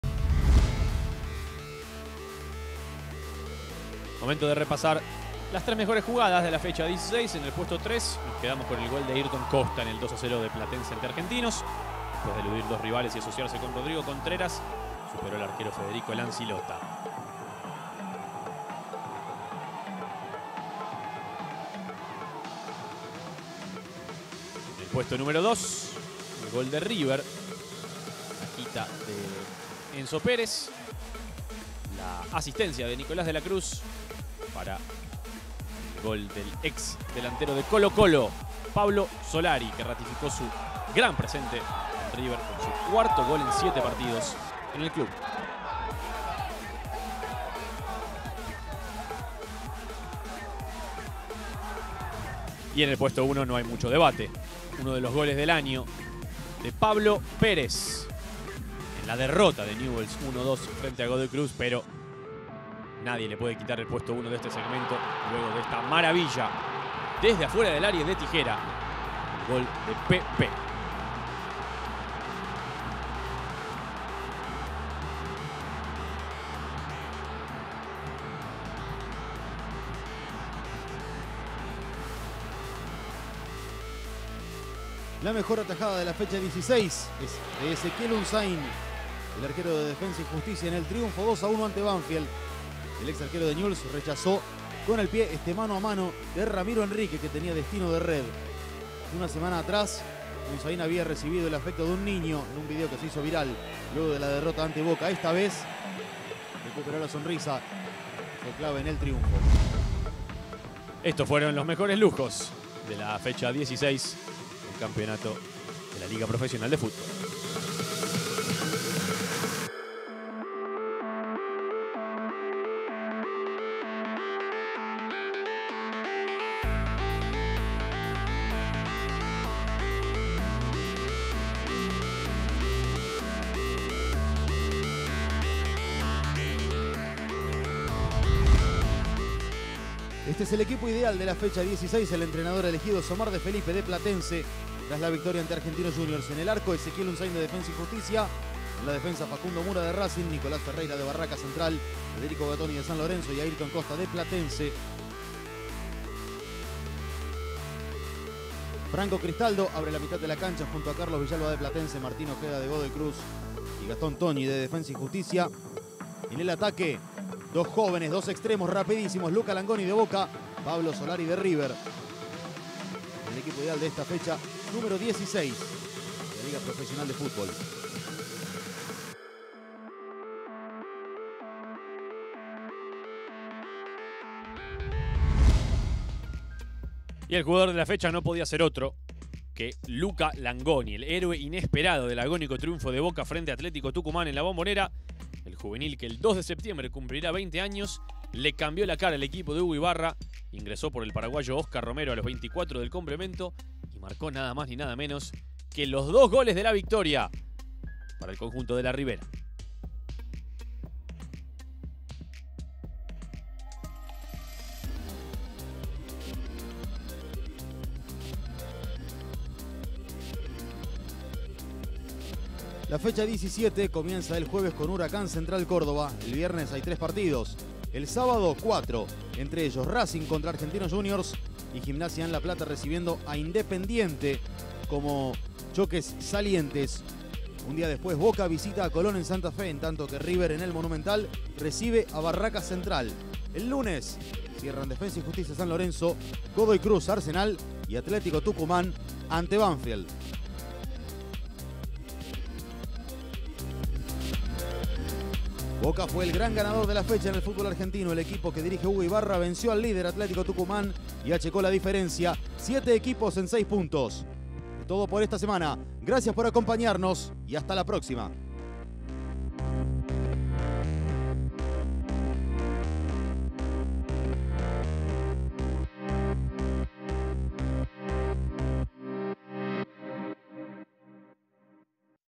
LPF TorneoBinance Resumen Fecha 16 | Los mejores momentos y más!
Mira el Magazine completo, el resumen de la fecha 16 en inglés y español, y disfruta de todos los partidos del Torneo Binance de la Liga Profesional de Fútbol argentino en VIVO y HD afuera de Argentina y Brasil

Mejores Momentos
3. Golazo de Ayrton Costa de Platense en el 2x0 contra Argentinos.
2. Gol de River del ex-jugador de Colo-Colo, Pablo Solari.
1. Uno de los golazos del año de Pablo Pérez para Newell's Old Boys de tijera.

Mejor Atajada
La mejor atajada de la Fecha 16 fue de Ezequiel Unsain, arquero de Defensa y Justicia.

Once Ideal de la Fecha 16
DT Omar De Felippe | Platense
Ezequiel Unsain  | Defensa Y Justicia
Facundo Mura  | Racing Club
Nicolás Ferreyra | Barracas Central
Federico Gattoni | San Lorenzo
Ayrton Costa | Platense
Franco Cristaldo | Huracán
Carlos Villalba | Platense
Martín Ojeda | Godoy Cruz
Gastón Togni | Defensa Y Justicia
Luca Langoni | Boca Juniors
Pablo Solari | River Plate

Jugador de la Fecha 16
Luca Langoni, fue la figura de la Fecha 16 con un doblete en la victoria sobre Atlético Tucumán en La Bombonera. Con solo 19 años entro en el minuto 70 y tuvo un partidazo para el Xeneize.